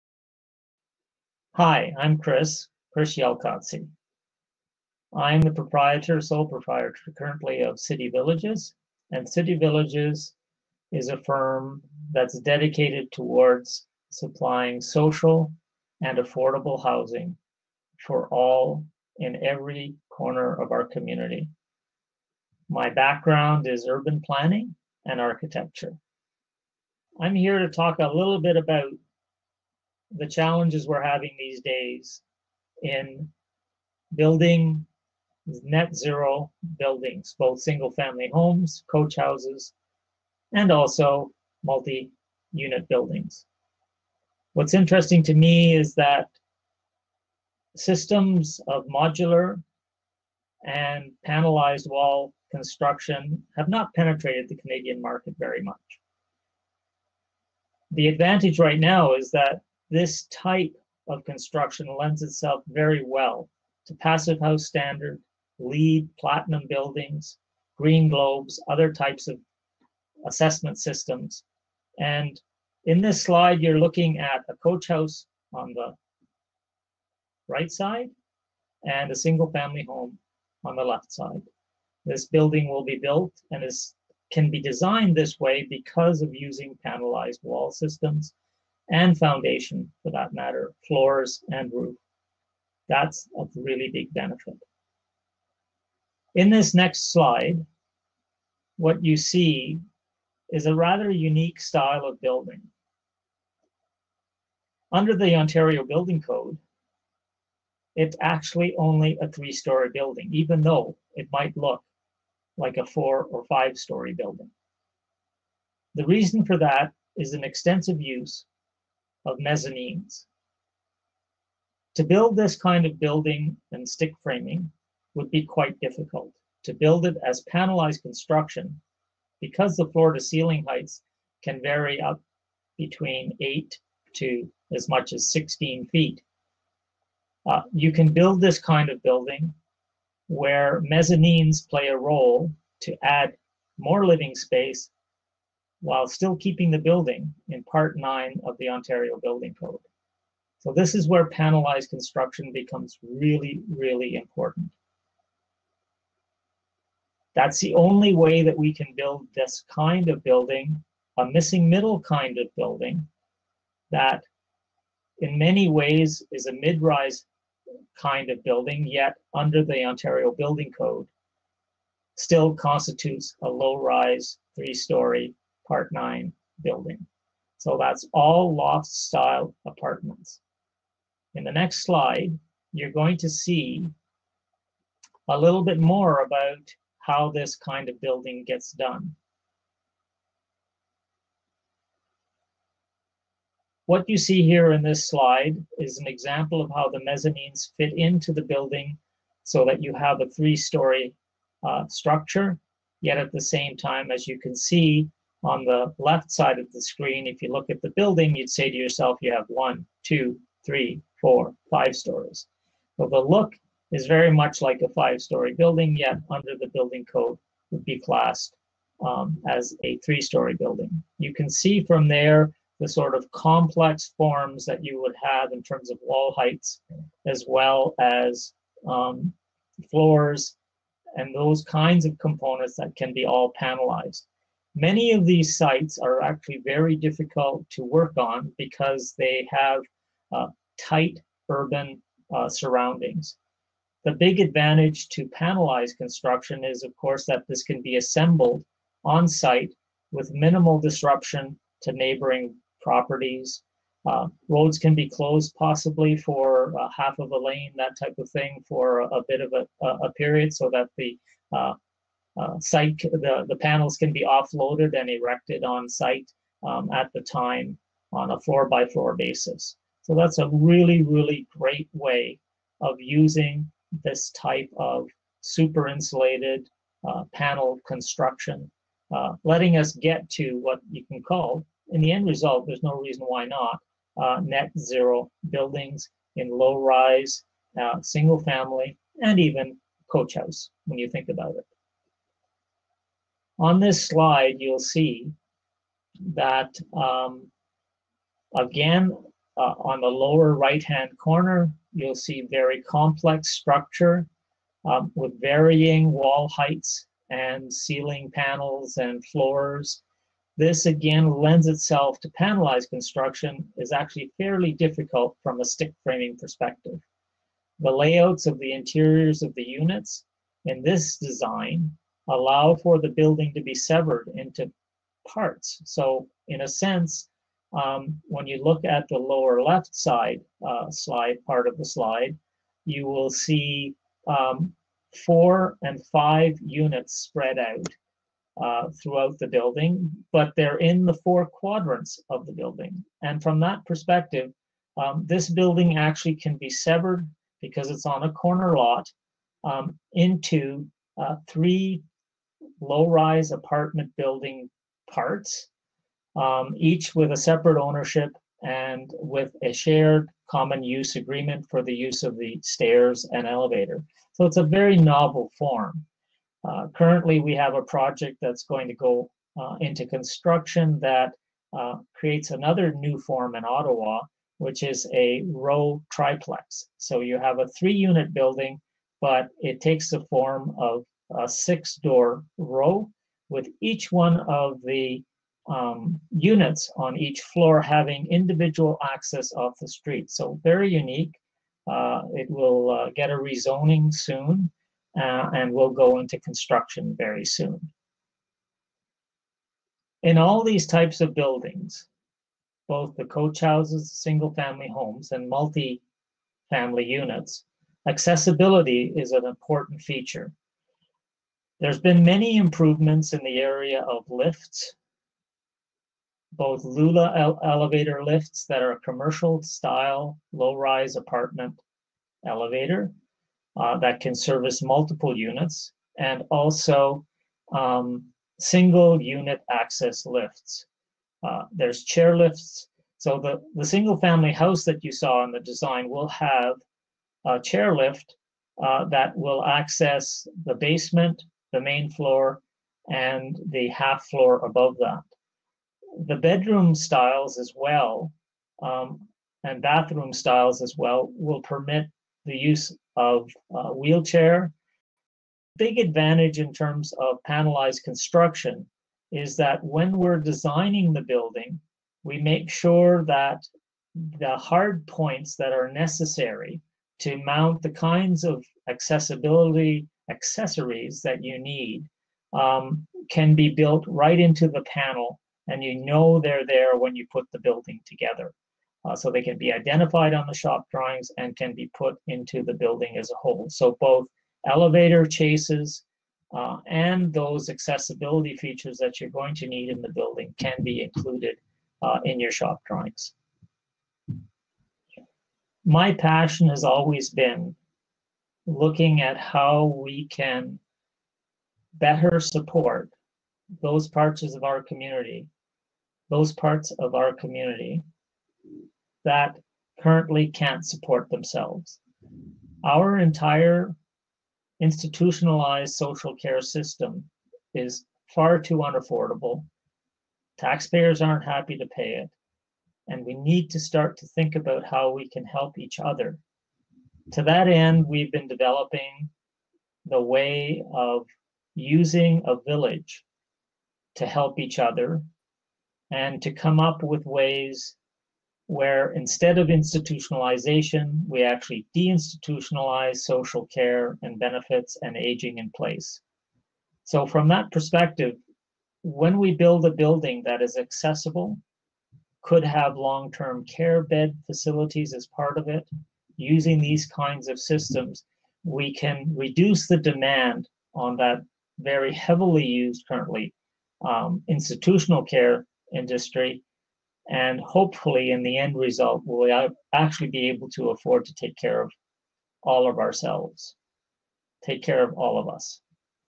Chris Jalkotzy: Harmonic System Flexibility
Chris Jalkotzy is the Founder of City Villages.
In this video, Chris talks about the flexibility of the Feris Harmonic System.

Topics covered:
- Single and coach homes
- Loft-style apartments
- 1 bedroom and loft-style apartments